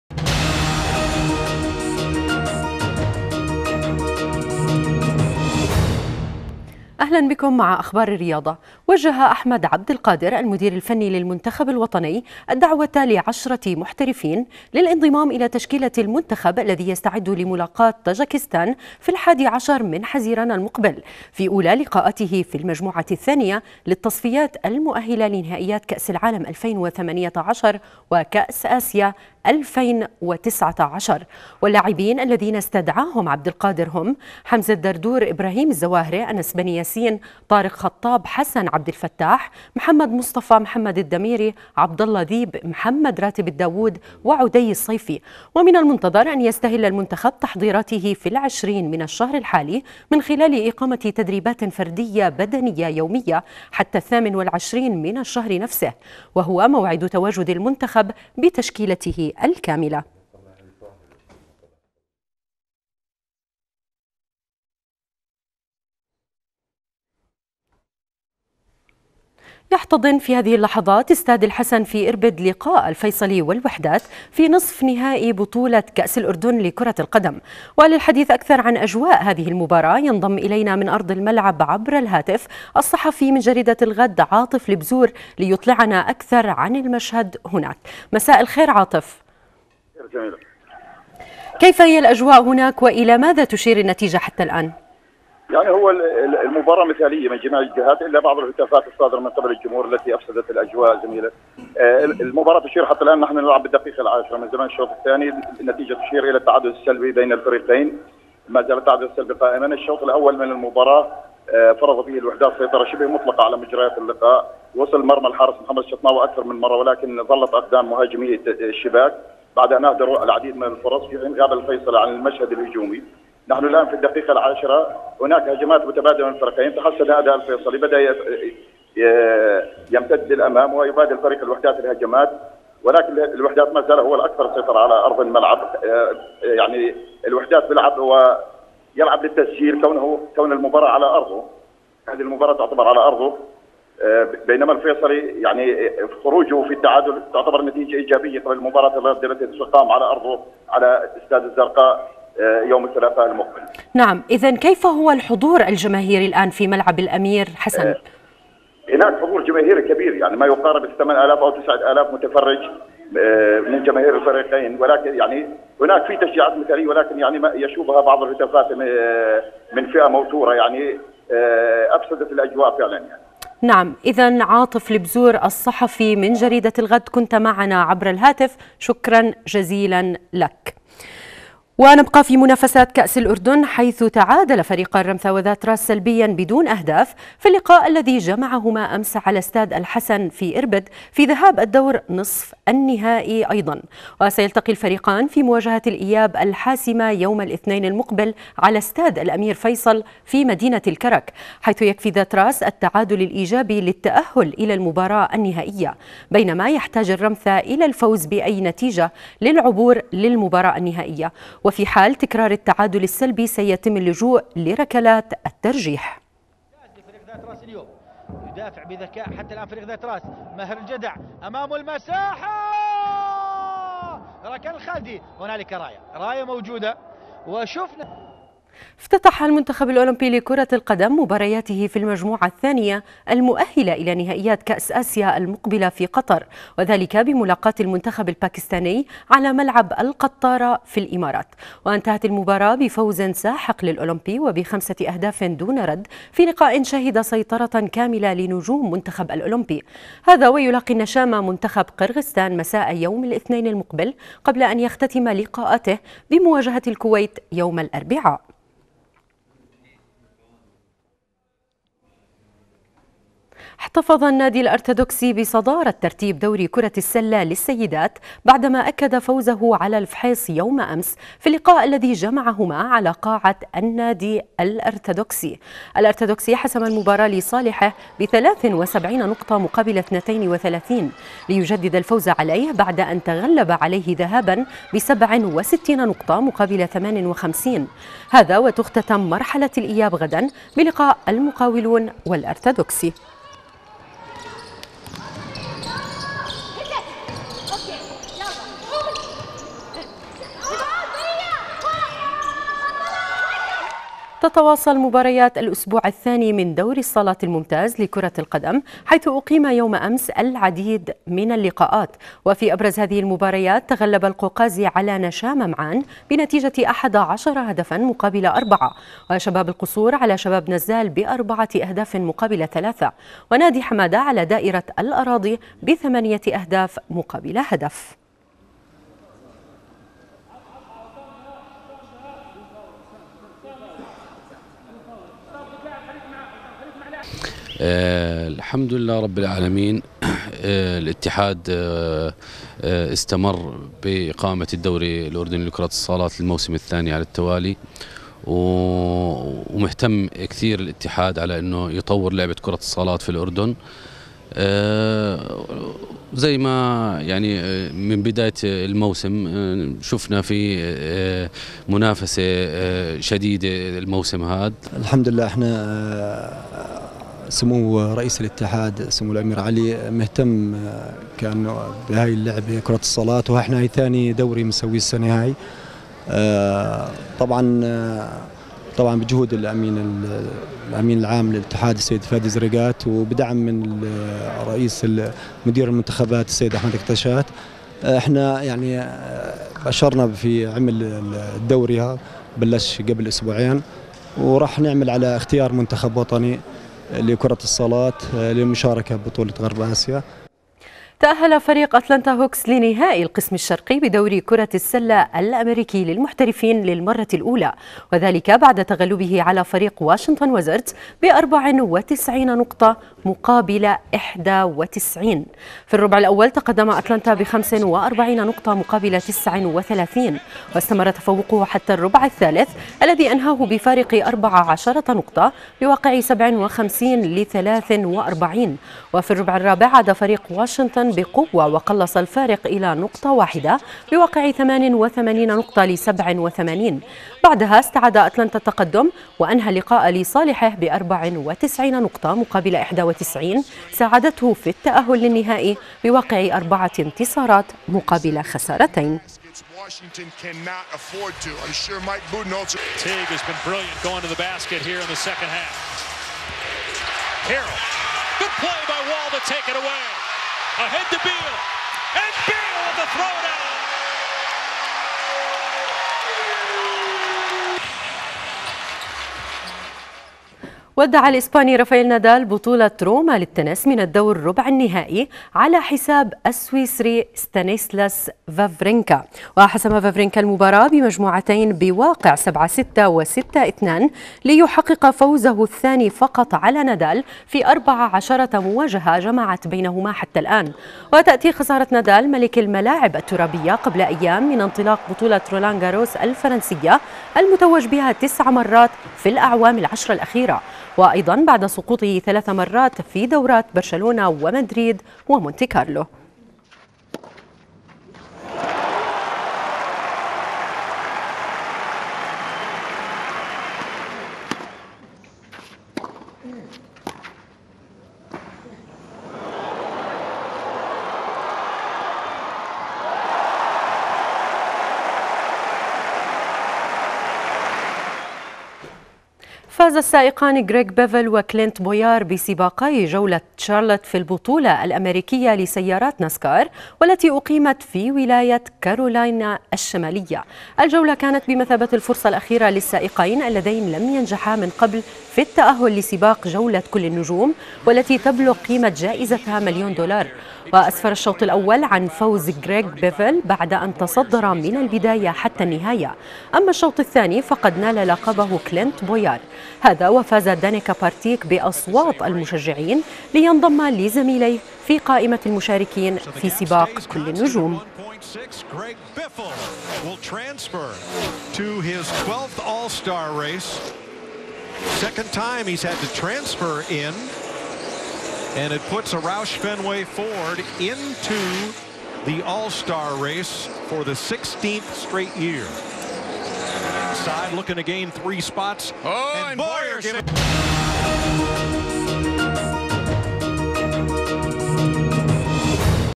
النشرة الرياضية 16-5-2015
النشرة الرياضية التي قدمتها الزميلة سميحة مجدلاوية ليوم 16/5/2015.